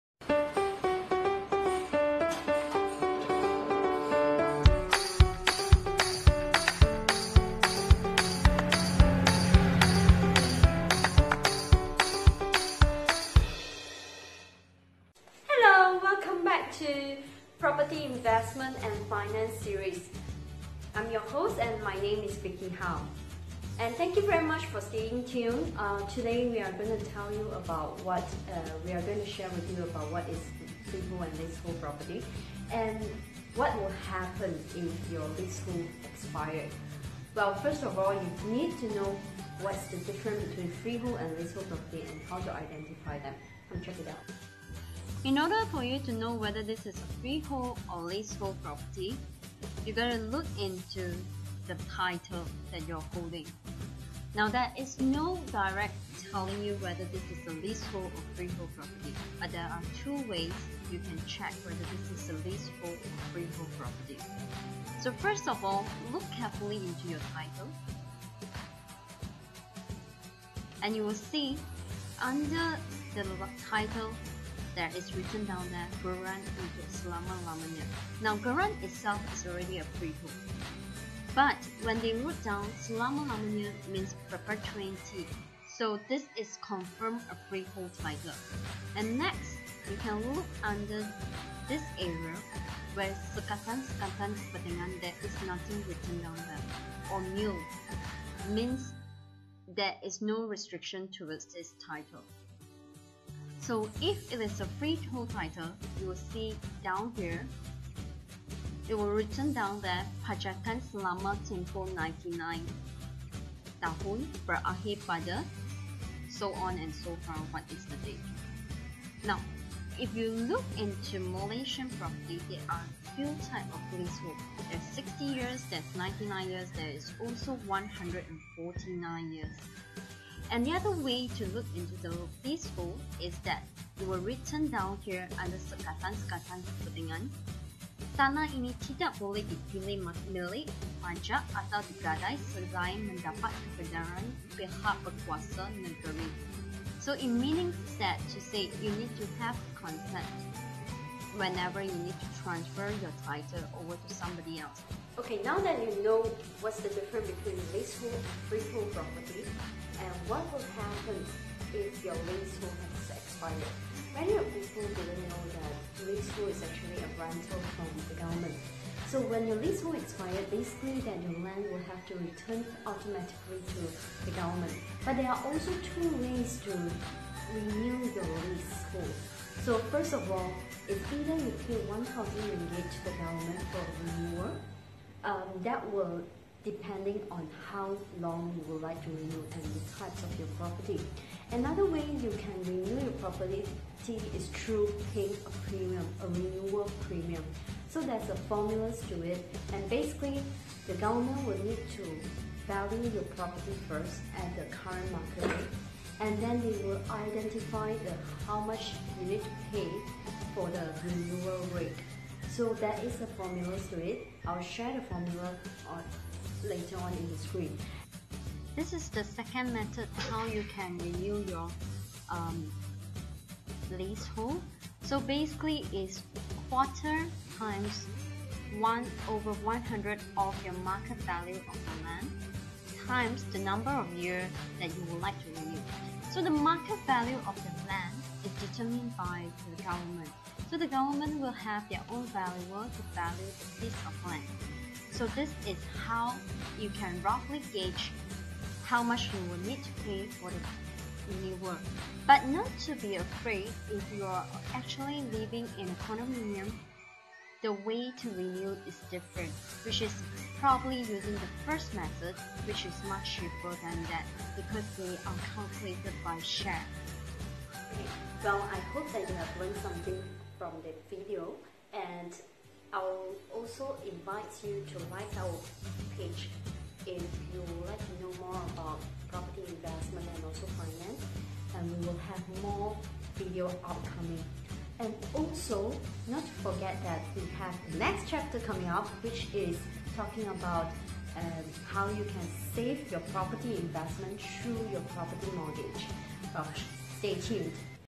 PIF series #02 How To Renew Leasehold Property
PIF series #02
Frequent asked question, leasehold property. When leasehold property have less than 45 years of lease, owner may find it hard to dispose the property due to the protential buyer not able to secure a loan from the bank. So, how can I renew my leasehold property?